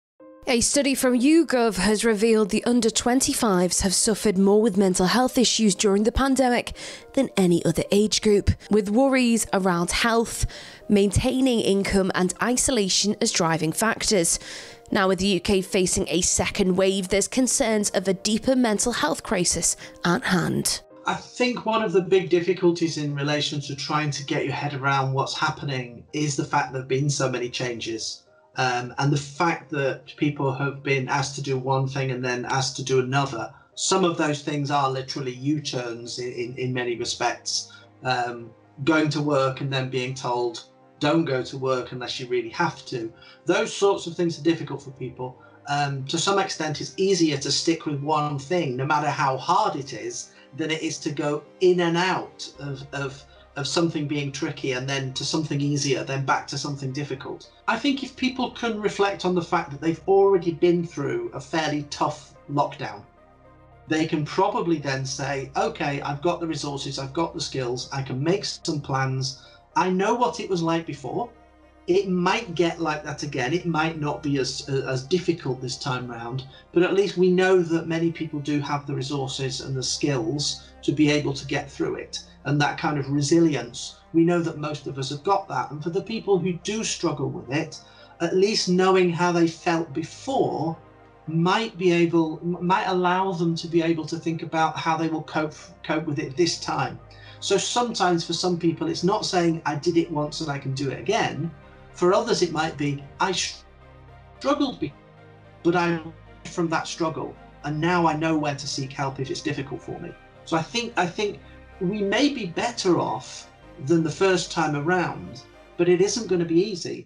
UGOV: Under 25s mentally suffering the most
Study for UGOV reveals under 25s have suffered more with mental health issues during the pandemic than any other age group, with worries around health, isolation and maintaining an income as driving factors.